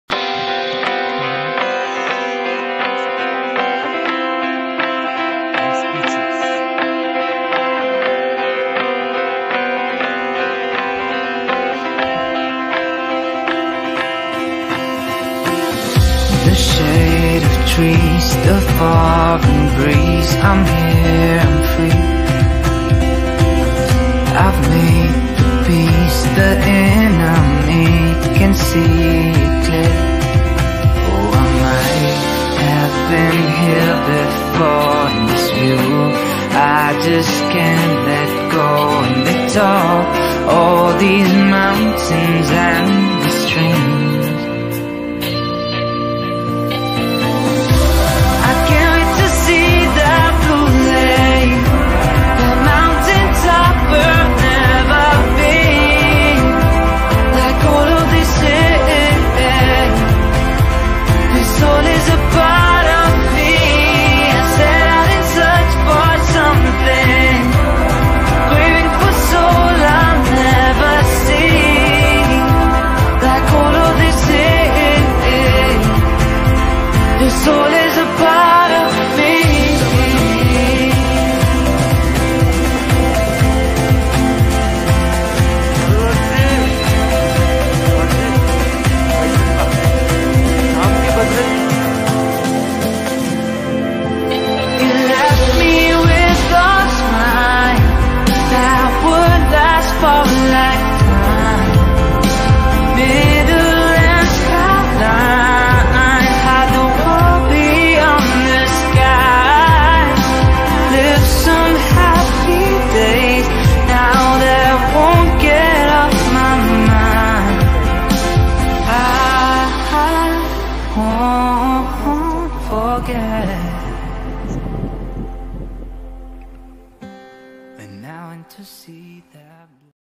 Mountains are calling | Tour of Arunchal Pradesh | Trailer
This video is all about Arunchal Pradesh tour. It was 3days tour . In which i visited Bomdila, Tawang, Sela pass and experienced many exciting views over there.

Arunchal vlog coming soon..

(Music used in this video are solely promote to artist art)